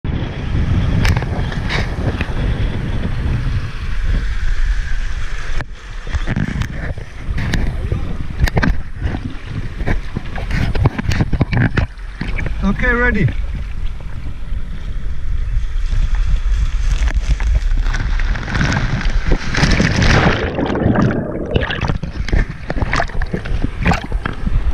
Speedrutsche Wasserrutsche AquaPark Tia Heights Makadi Bay
Hier möchte ich euch die Wasserrutschen im Aqua Park des Hotel Tia Heights in Makadi Ägypten vorstellen. Weitere Wasserrutschen findet Ihr in meinen anderen Videos.